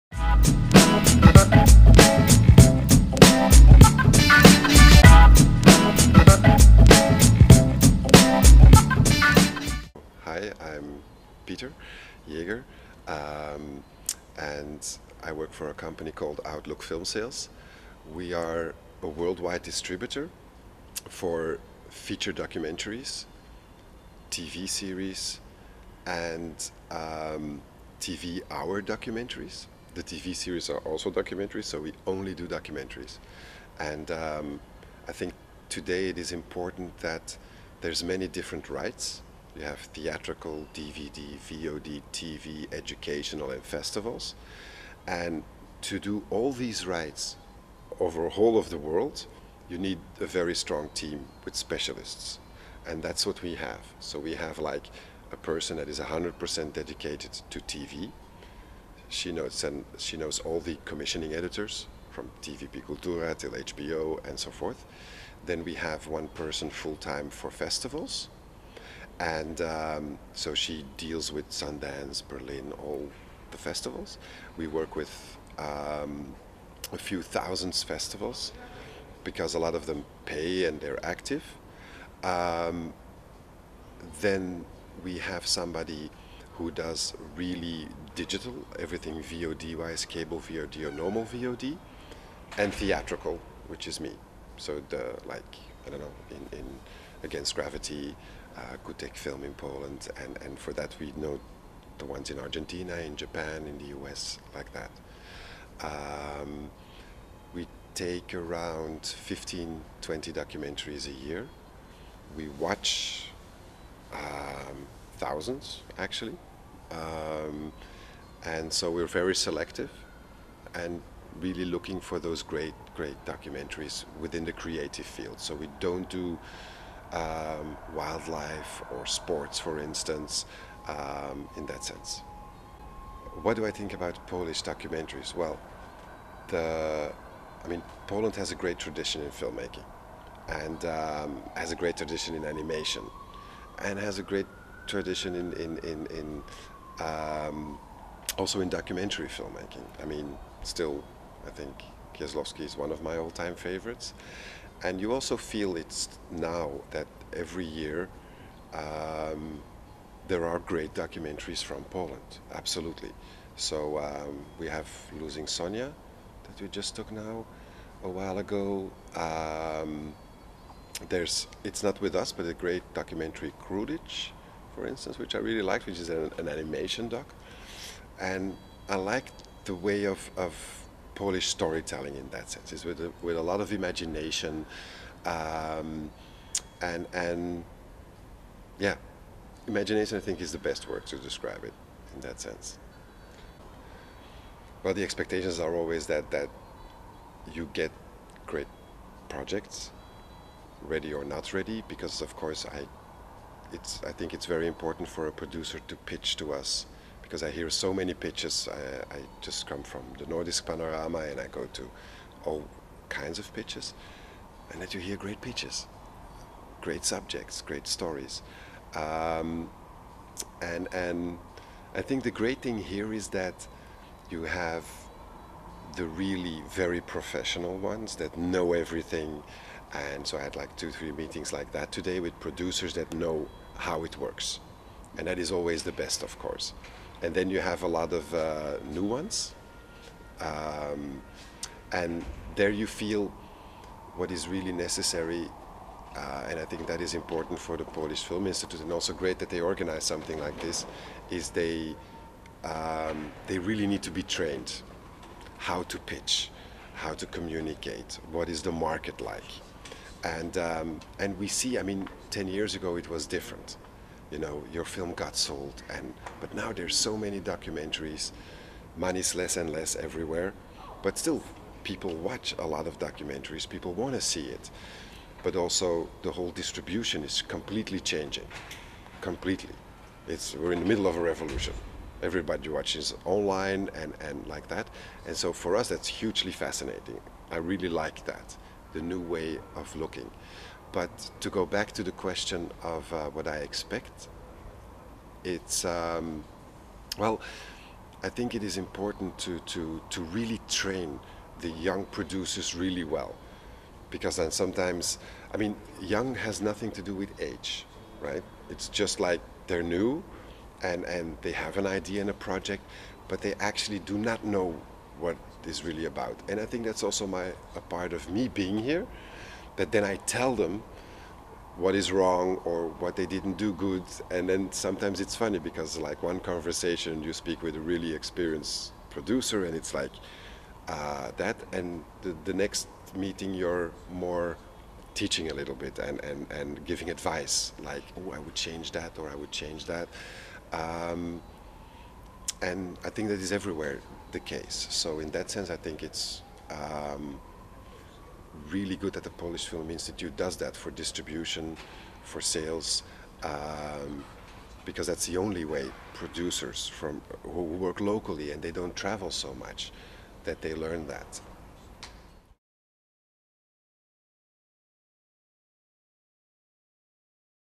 Rozmowa z Peterem Jagerem
Rozmowa z Peterem Jagerem, agentem sprzedaży z Autlook Filmsales, jednym z gości Polish Films Internetional Audience, które odbywały się w Warszawie w dniach 25-27 września 2012.